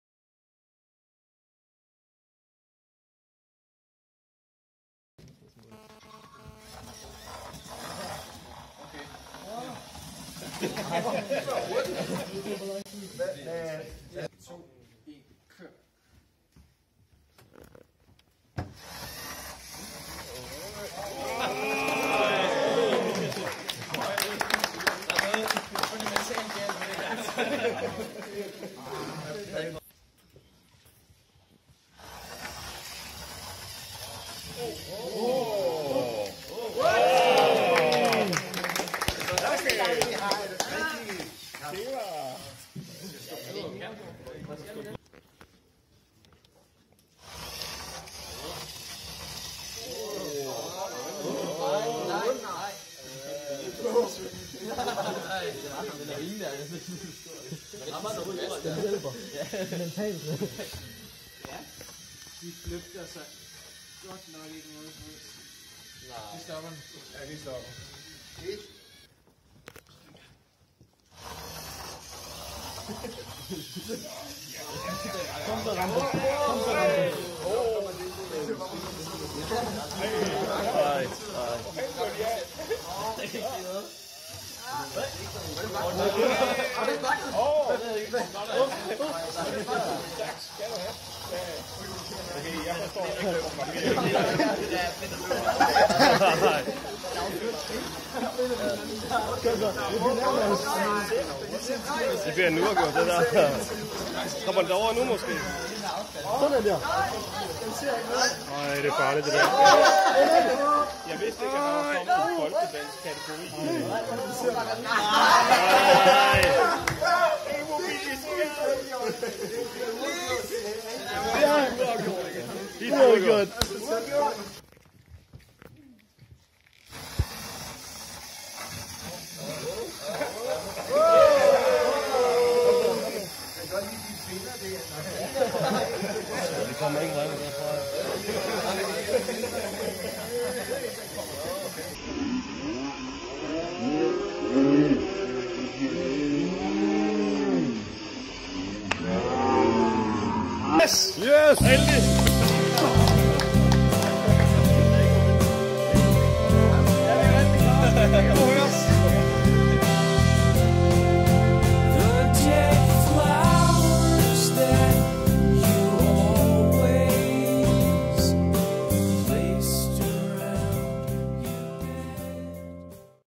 SG ved World Robot Olympiad 2017
Den danske afvikling af World Robot Olympiad på Aarhus Universitet, oktober 2017. International finale afvikles i Costa Rica. Kommer vi med?